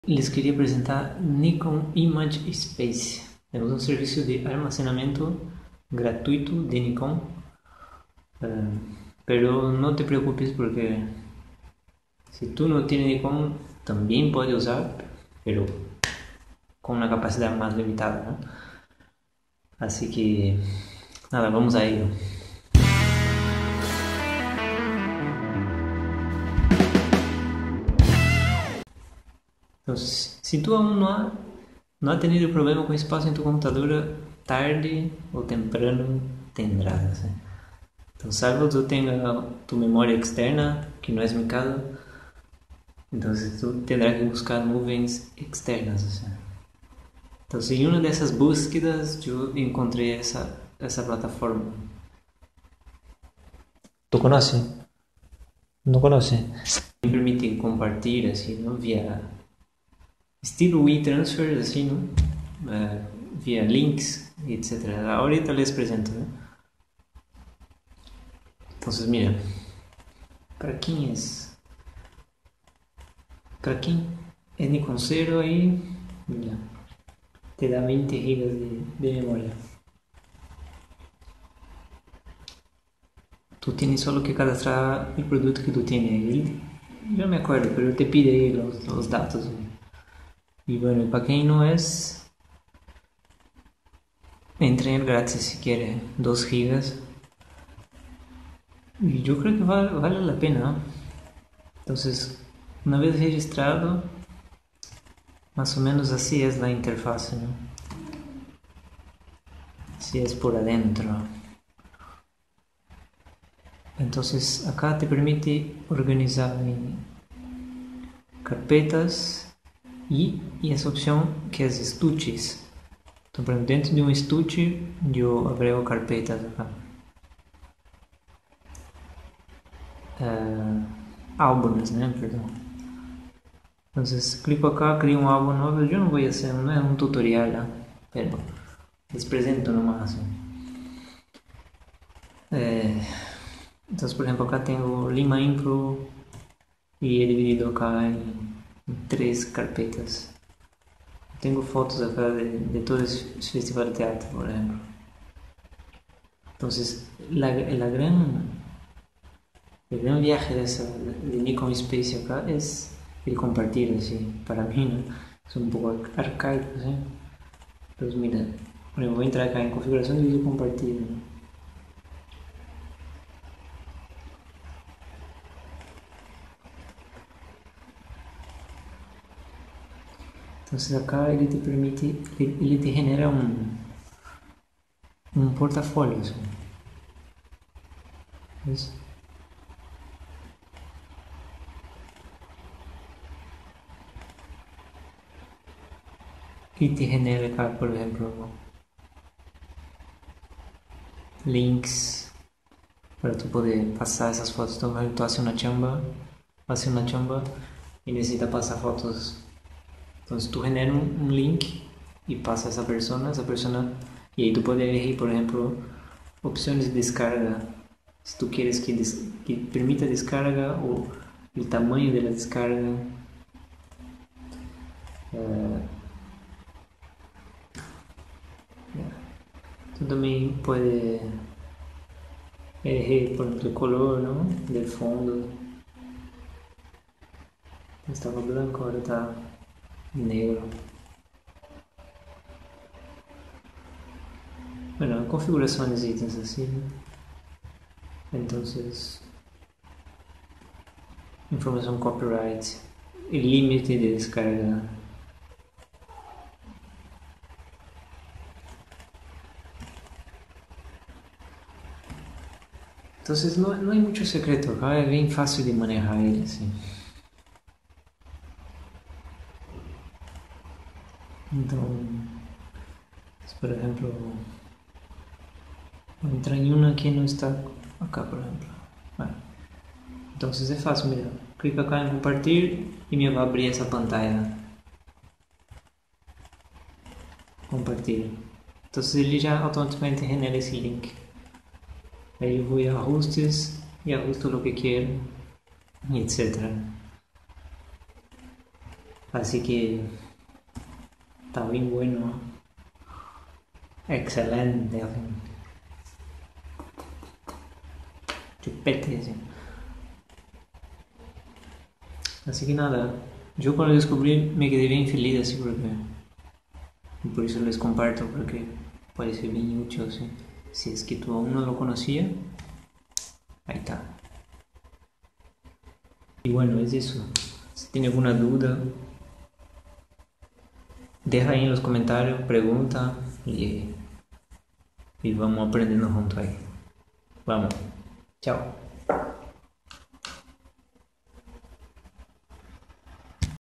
Vlog 3: NIKON IMAGE SPACE: Almacenamiento de fotos GRATUITO para nikonistas (y no nikonistas).
¡Gracias! En ese video introduzco Nikon Image Space, un servicio de almacenamiento gratuito de Nikon para nikonistas (20 Gb) y no nikonistas (2 Gb). Ademas ofrece un servicio de transferencia que te permití compartir sus fotos, facilitando el envío de sesiones a sus clientes.
Soporta JPG y MP4.

¿Necesitas una página web?